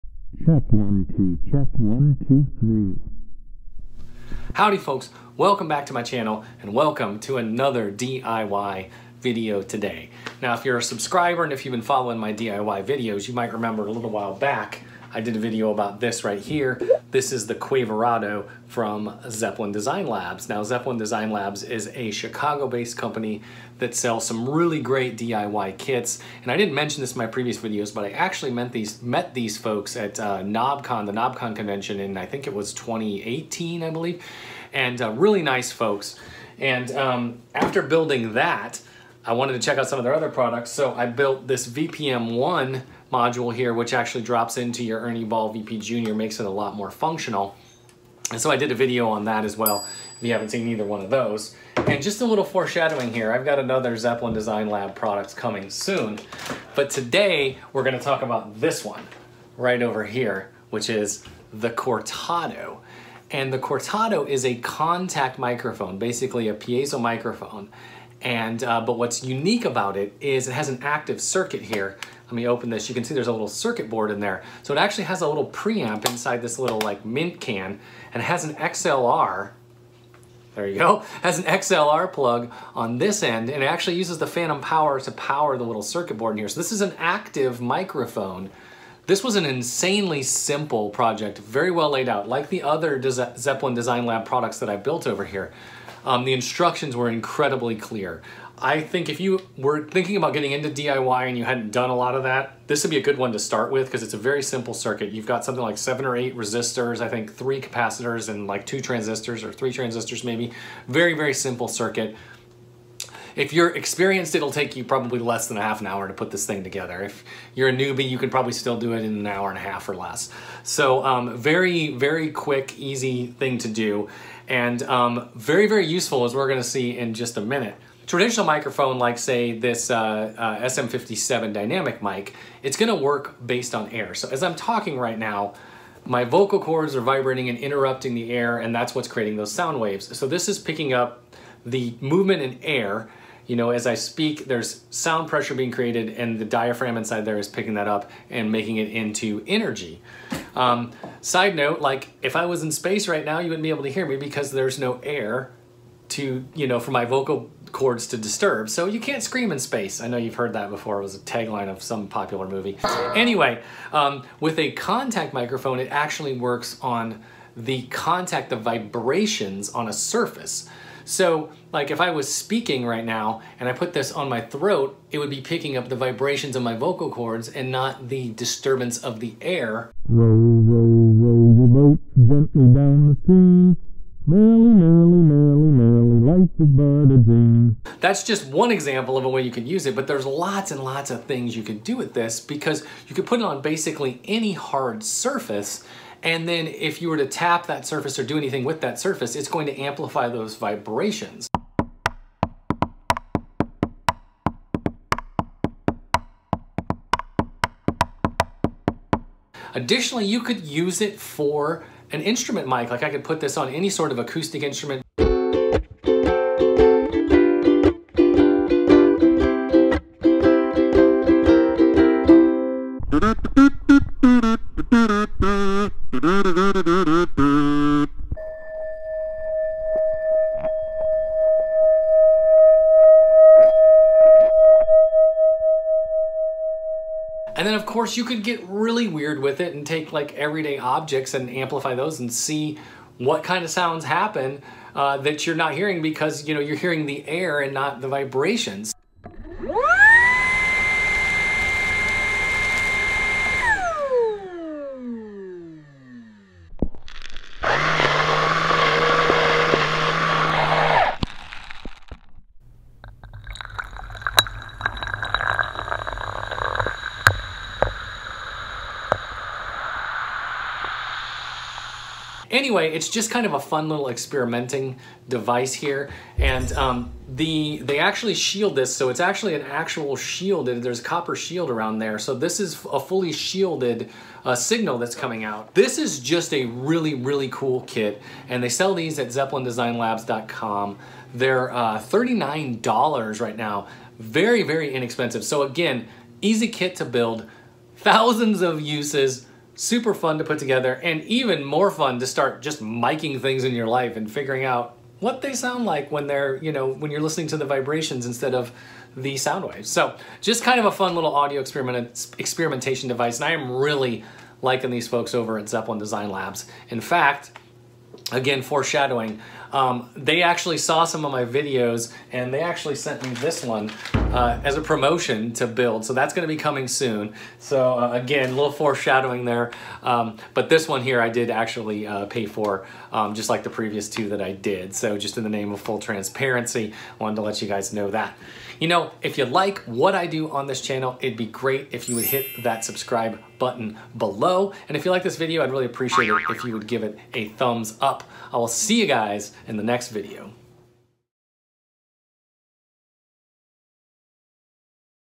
Zeppelin Design Labs Cortado DIY Kit (contact microphone)
This is my third kit from @zeppelindesign (ZDL) in Chicago. The Cortado is an active contact microphone which uses phantom power. It can be used for a variety of things ranging from amplifying acoustic instruments to recording everyday objects. Like the other ZDL products I've built, this kit was very well thought out and had great instructions.